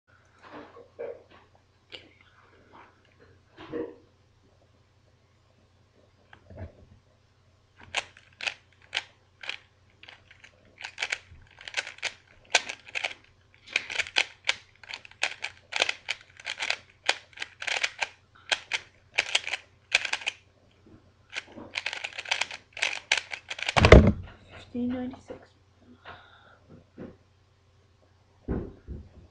15.96 solve on my GuHong
A solve that I did on my dayan guhong cube that took me 15.96 seconds to complete